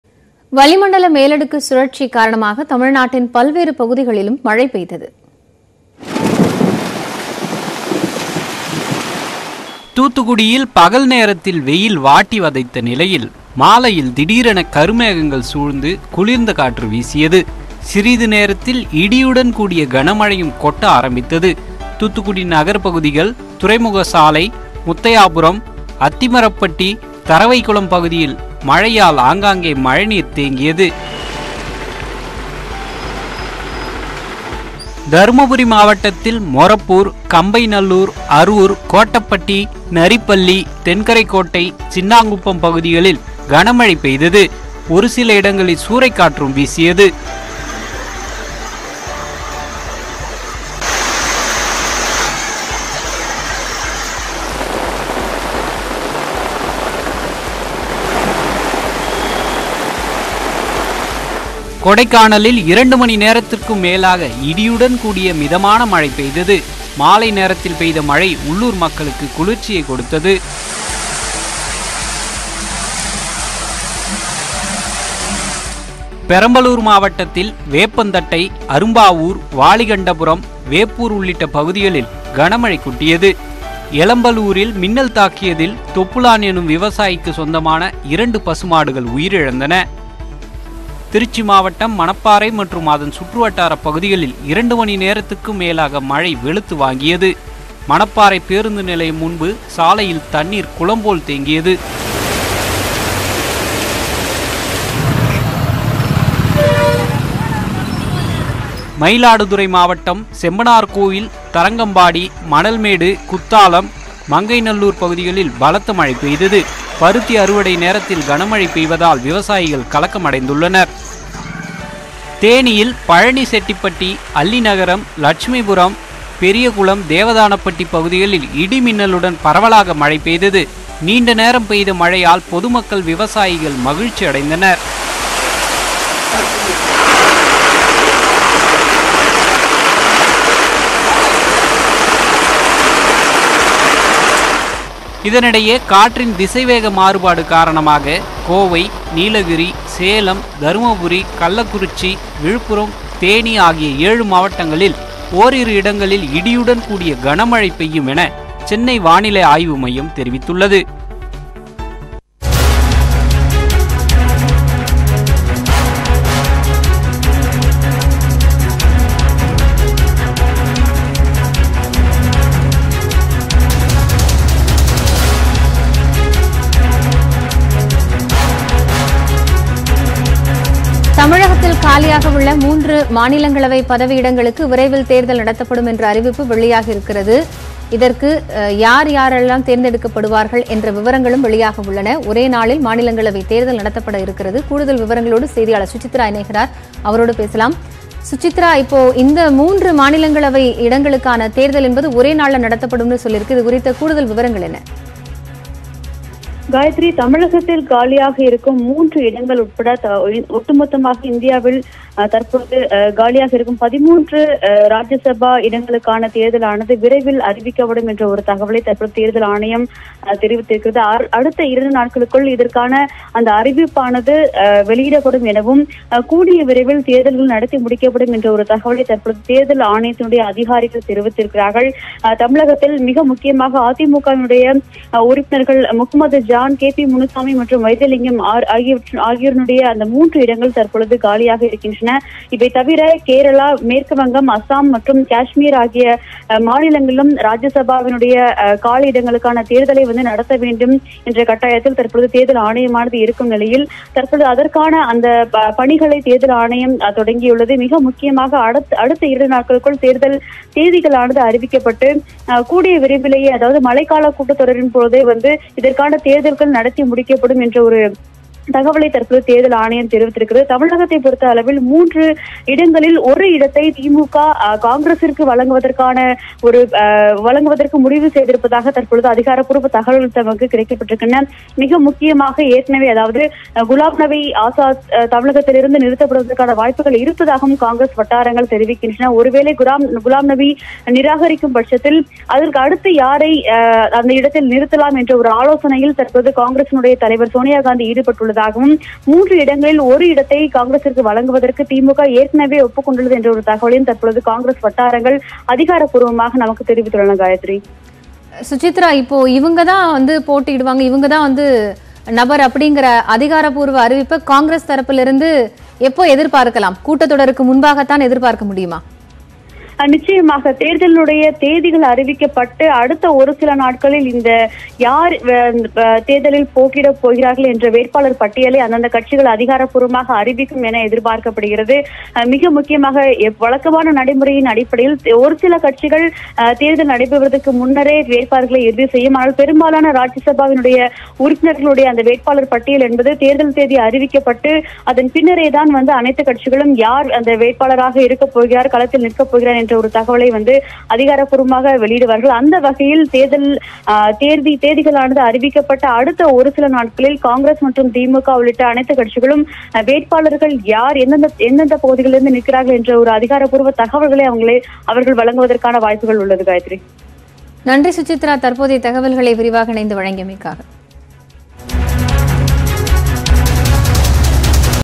Rain News Tamil | தூத்துக்குடியில் கனமழை - முத்தையாபுரம், தருவைகுளத்தில் தேங்கிய மழைநீர்
Rain News Tamil | வளிமண்டல மேலடுக்கு சுழற்சி காரணமாக தமிழ்நாட்டின் பல்வேறு பகுதிகளிலும் மழை பெய்தது | தூத்துக்குடியில் இடியுடன் கனமழை | தூத்துக்குடியில் கனமழை - முத்தையாபுரம், தருவைகுளத்தில் தேங்கிய மழைநீர் | இது குறித்த கூடுதல் தகவல்களுடன் உங்கள் நியூஸ்18 தமிழ்நாடு